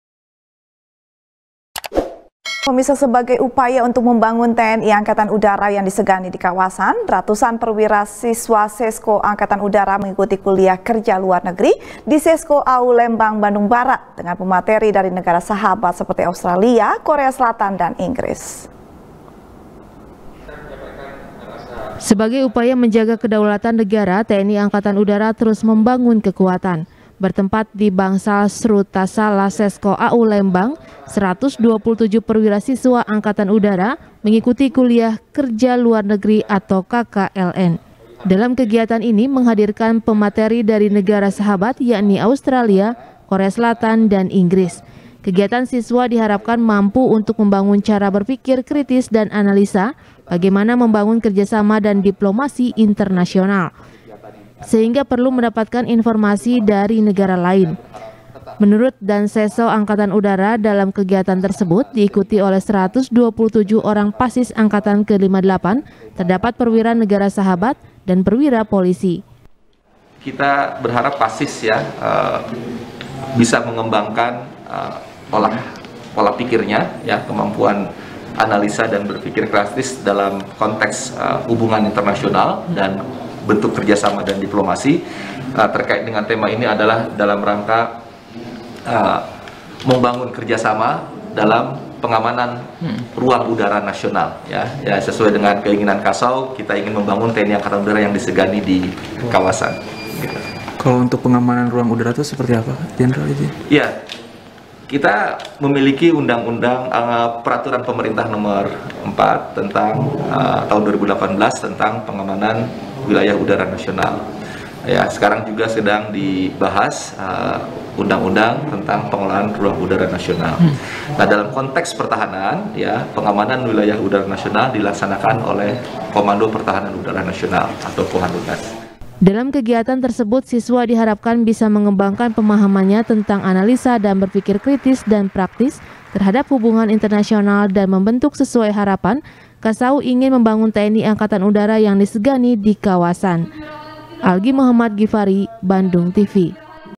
Sesko AU Hadirkan Pemateri Dari Tiga Negara Dalam KKLN | SBR | BANDUNGTV
SBR | BANDUNGTV | FARHAT MUMTAZ | Sebagai Upaya Membangun Tni Angkatan Udara Yang Disegani Di Kawasan. Ratusan Perwira Siswa Sesko Angkatan Udara Mengikuti Kuliah Kerja Luar Negeri, Di Sesko Au Lembang Bandung Barat, Dengan Pemateri Dari Negara Sahabat Seperti Australia, Korea Selatan, Dan Inggris.